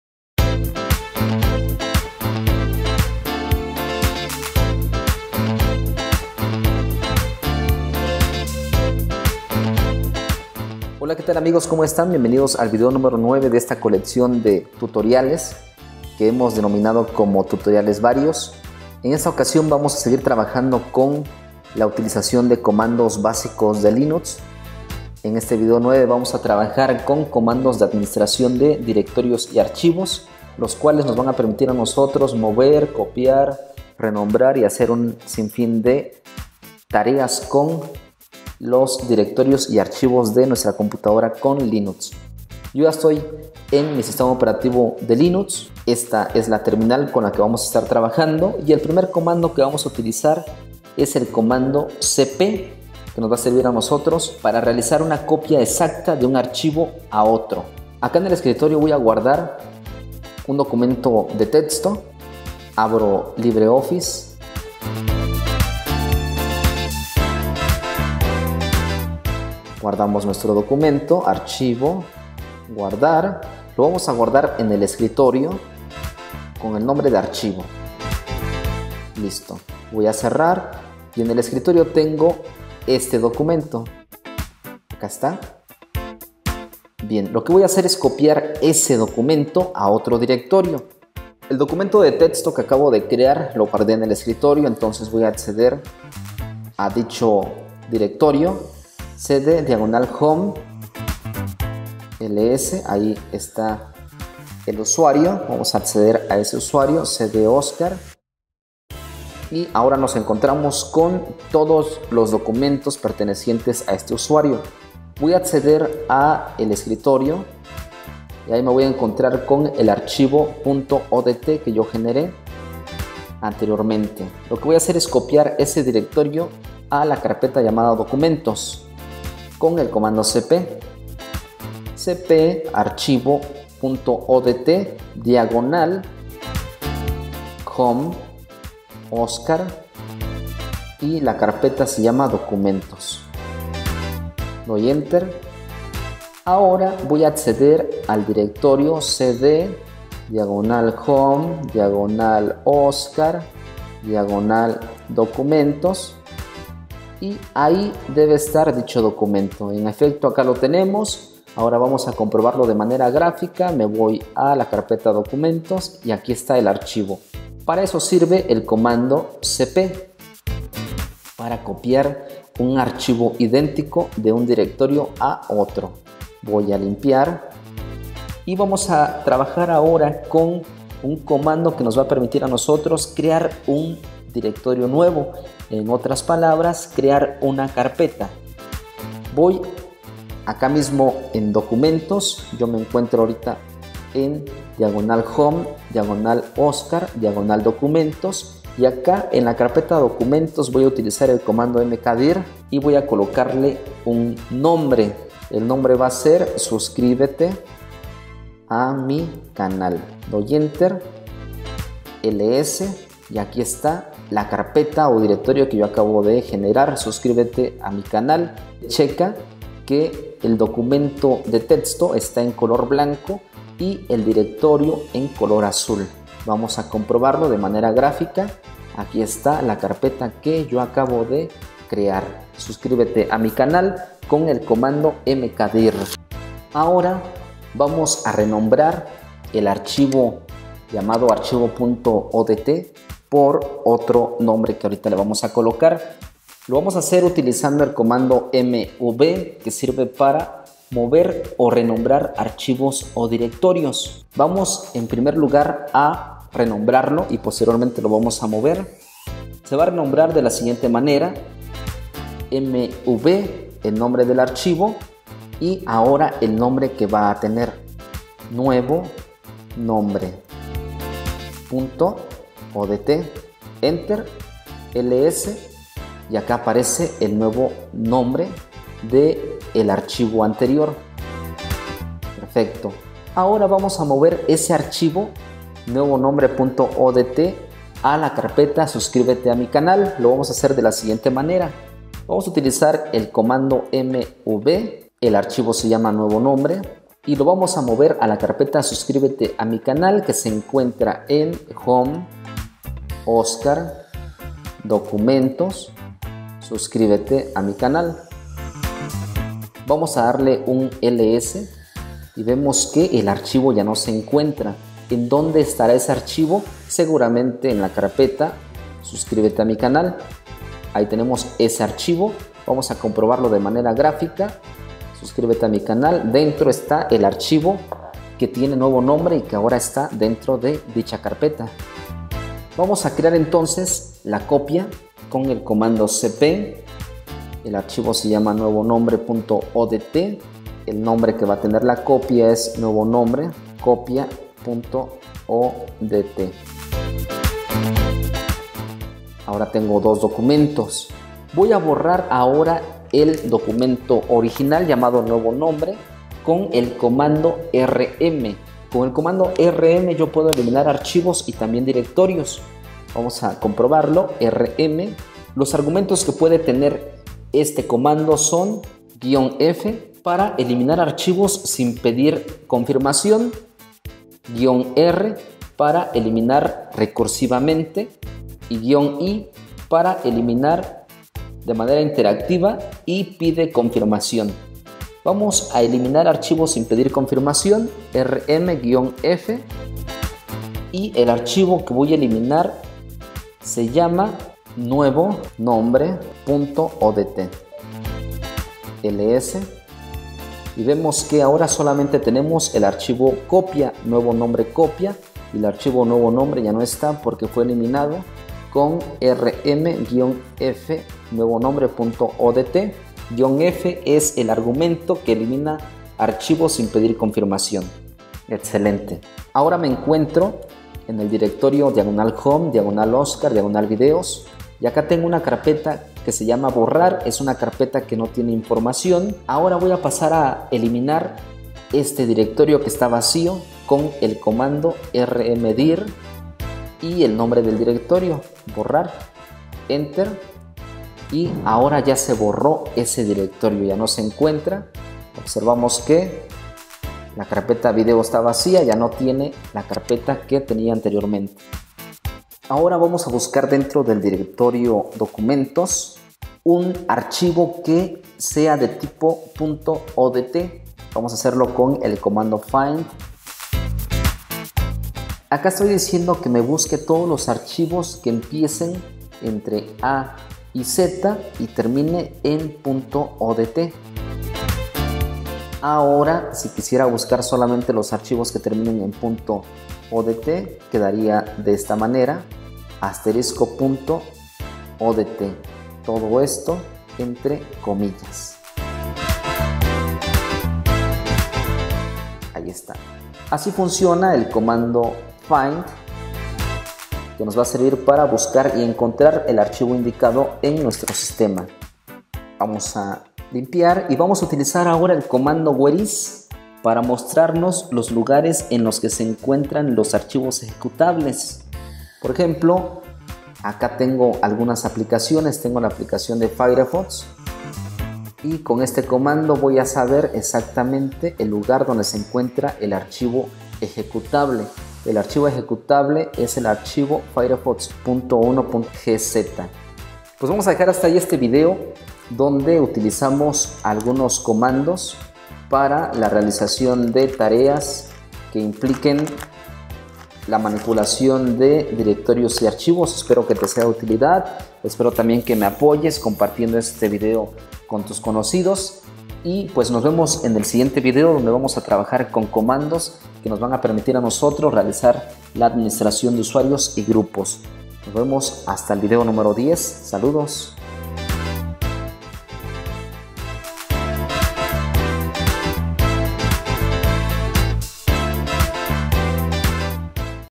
Video 9. Tutoriales Varios | Comandos en Linux para el manejo de directorios y archivos
cp, mkdir, mv, rm, rmdir, find, whereis